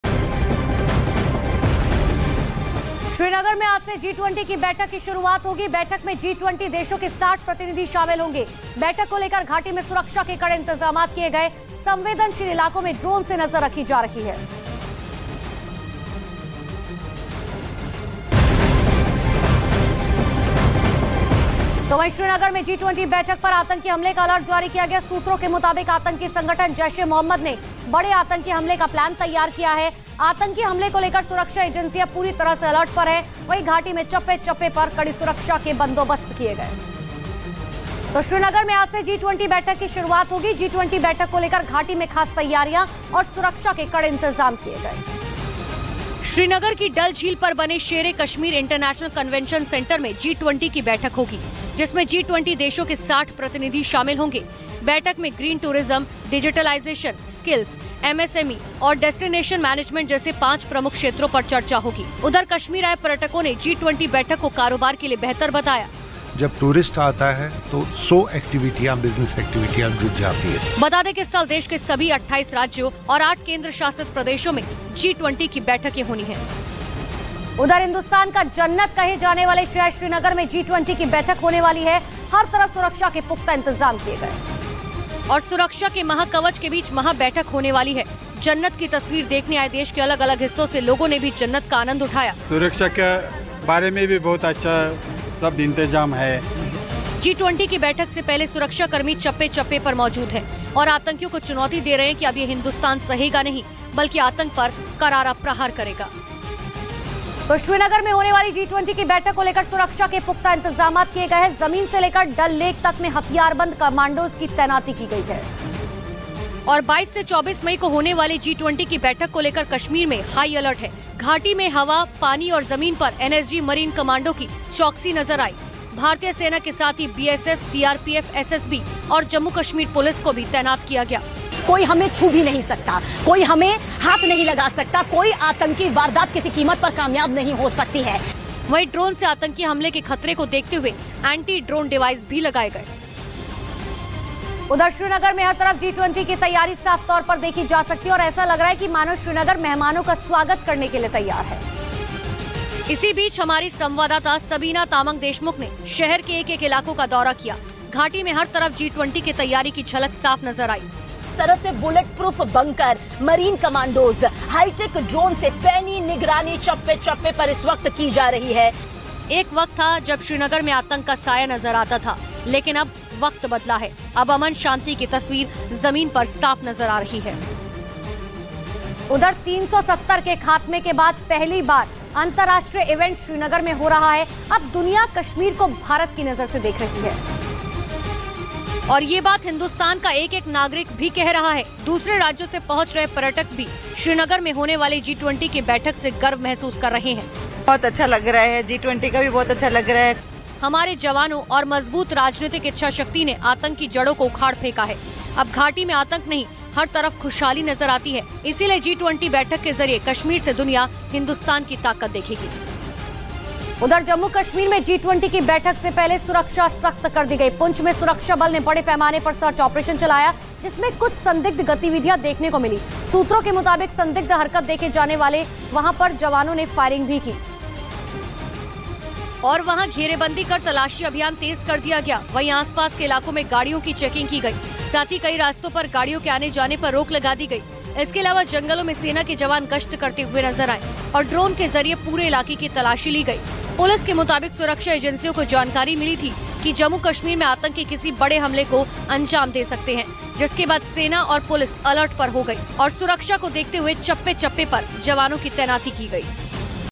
G20 In Kashmir : Srinagar में आज से G20 की बैठक, 60 देशों के प्रतिनिधि होंगे शामिल | News18
G20 meeting will start in Srinagar from today Special preparations and strict security arrangements have been made in the valley regarding the G20 meeting. The G20 meeting will be held at the Sher-e-Kashmir International Convention Center on Dal Lake in Srinagar.
श्रीनगर में आज से G20 बैठक की शुरुआत होगी G20 बैठक को लेकर घाटी में खास तैयारियां और सुरक्षा के कड़े इंतज़ाम किए गए हैं। श्रीनगर की डल झील पर बने शेर-ए-कश्मीर इंटरनेशनल कन्वेंशन सेंटर में G20 की बैठक होगी